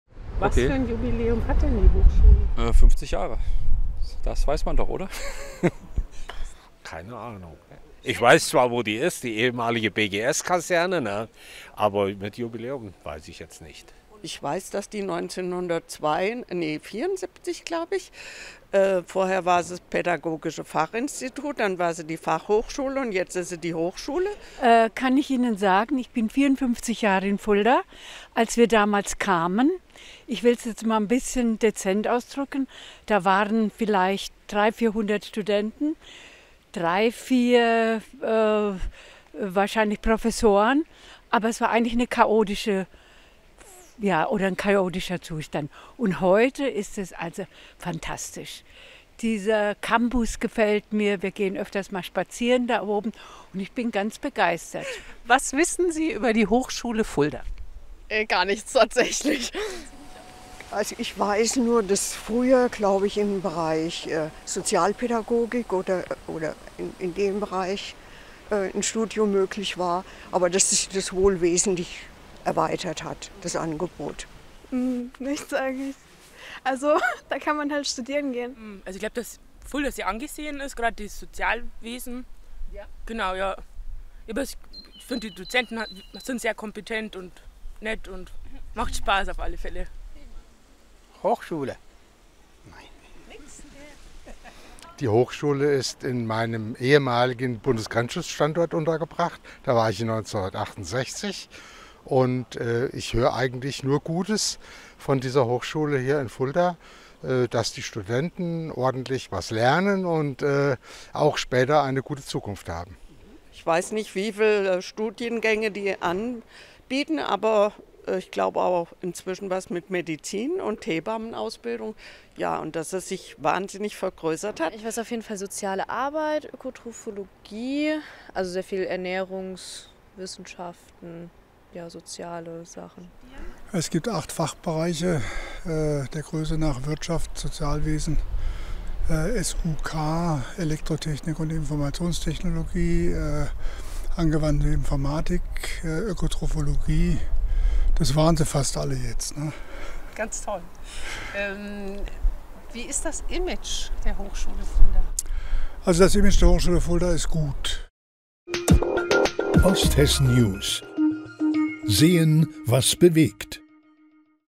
Wissen über 50 Jahre Hochschule Fulda - noch viele "Ahnungslose" unterwegs
Mit 50 Jahren steht man heutzutage mitten im Leben, meist auch auf dem Höhepunkt seiner beruflichen Laufbahn - durchaus noch mit Luft nach oben. Auf ein erfolgreiches halbes Jahrhundert blickt in diesem Jahr auch eine in Fulda fest etablierte Bildungsinstitution zurück, die trotz bester Auslastung, stetigem Wachstum und vieler positiver Schlagzeilen für etliche Einheimische noch ein ziemliches Rätsel zu sein scheint. Bei unserer kürzlichen Straßenumfrage (natürlich nicht repräsentativ!) taten sich bei den meisten Befragten große Bildungslücken auf. Seit wie vielen Jahren existiert die Hochschule Fulda? Was kann man denn da studieren? Und wie viele Studierende gibt es aktuell?
Quelle Vorschaubild: Hochschule Fulda, Nicole Heinisch, Johannes Ruppel